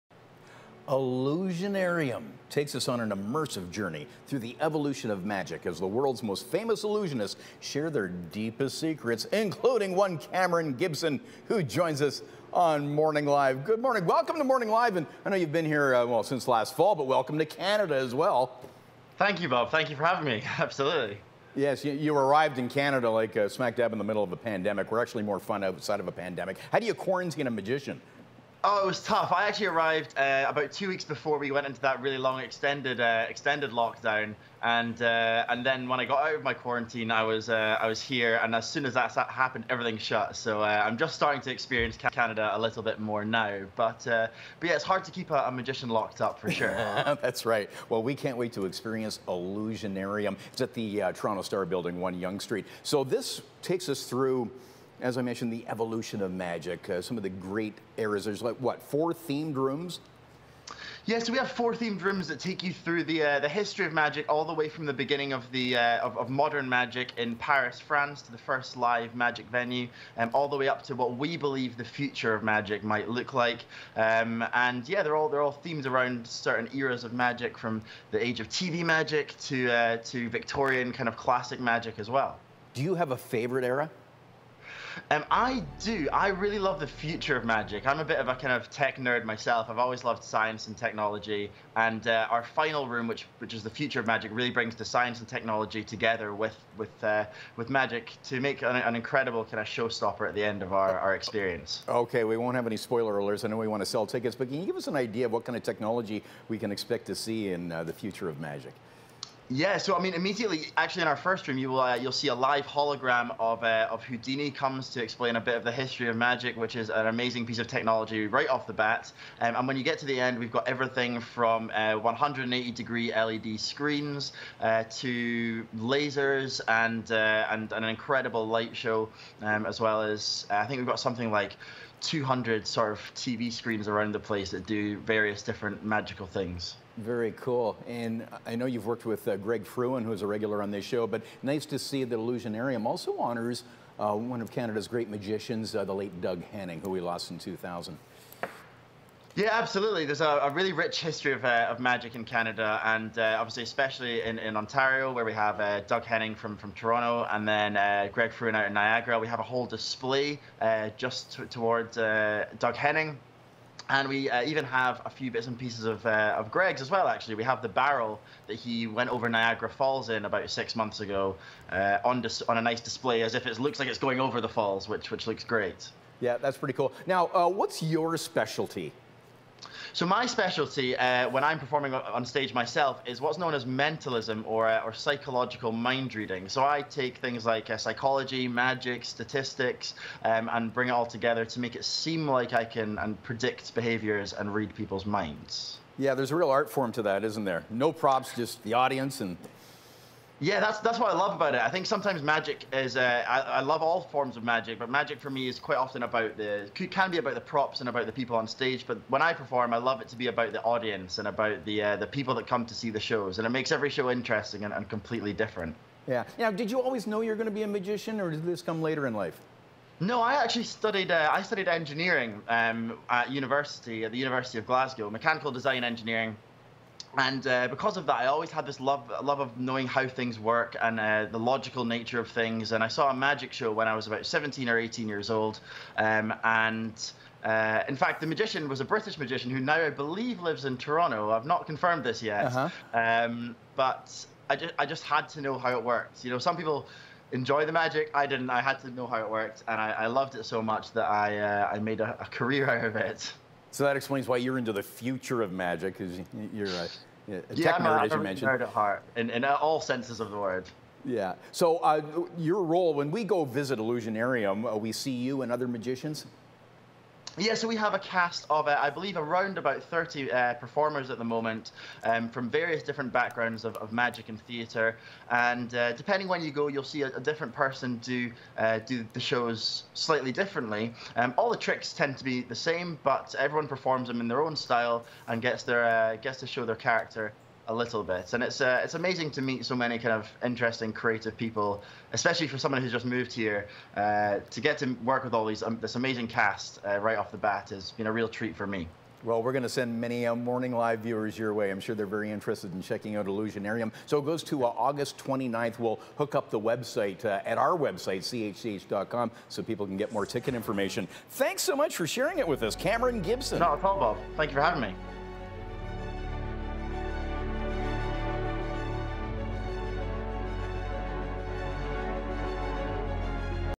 Illusionarium
Illusionarium takes us on an immersive journey through the evolution of magic as the world's most famous illusionists share their deepest secrets including Cameron Gibson who joined us this morning.